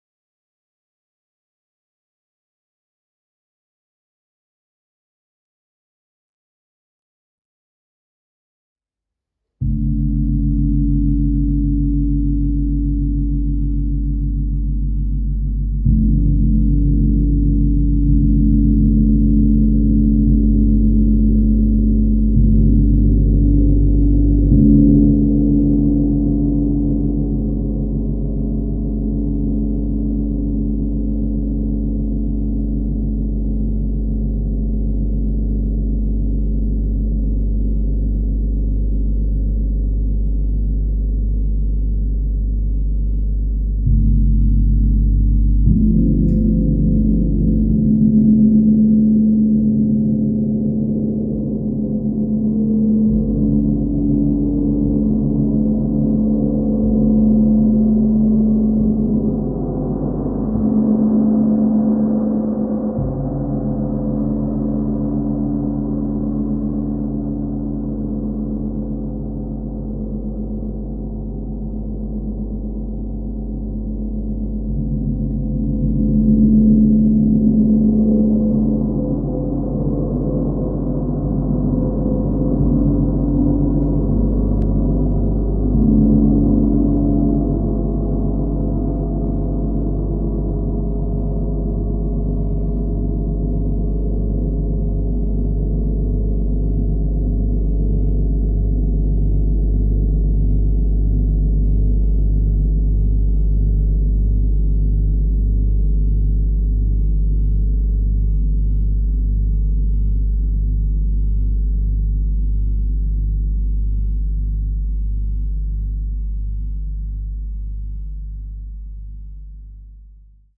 Gongzeit Rock Gong 46"/115cm
full sound... soft...present...big...A magnificent piece of a gong.